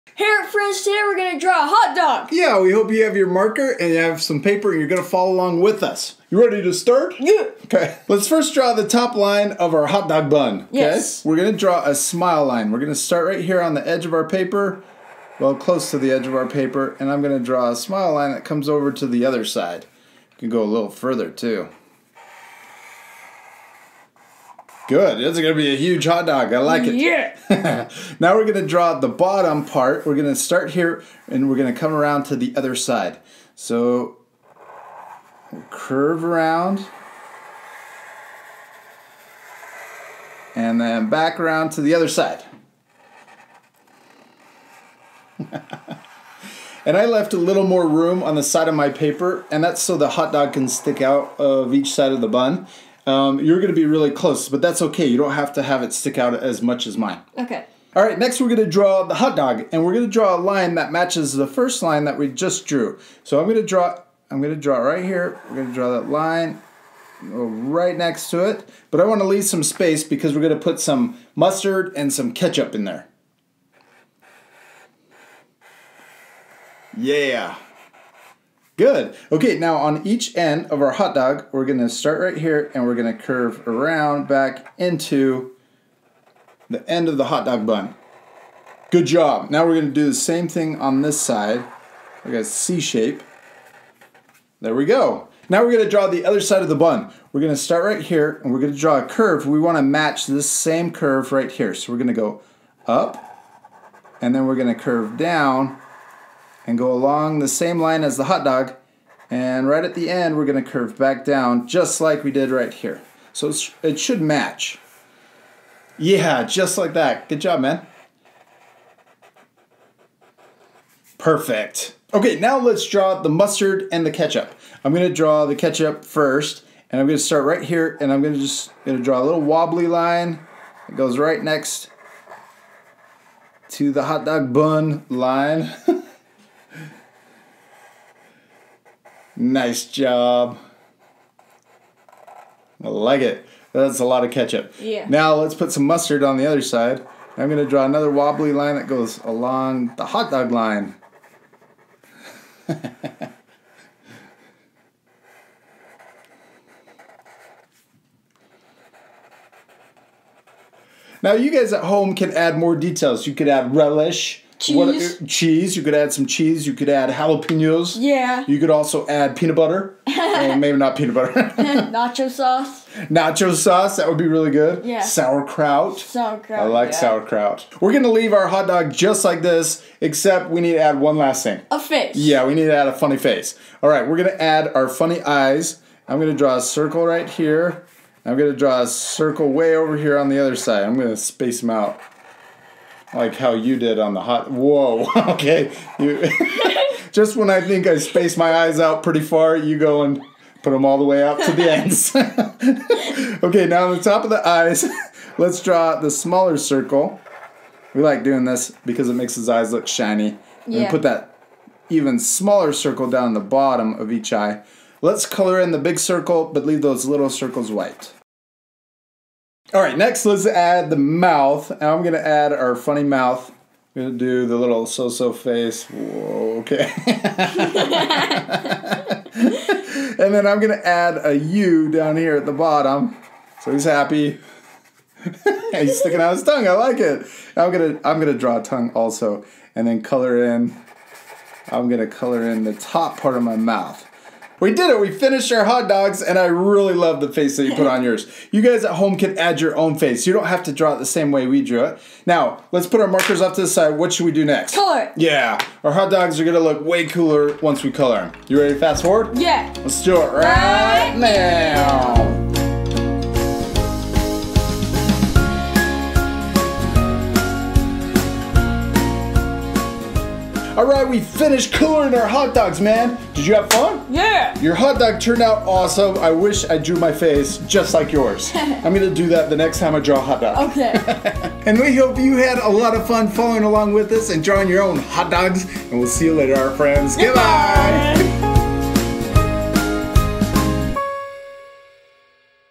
How To Draw A Funny Hot dog
Grab those markers and follow along with us. Today, we're learning how to draw a funny hotdog! If you just want to draw a hot dog, you can leave the funny face off. Also, remember to add more toppings and draw other funny food for his friends!

Would your kids like to have their art featured in our Monday video?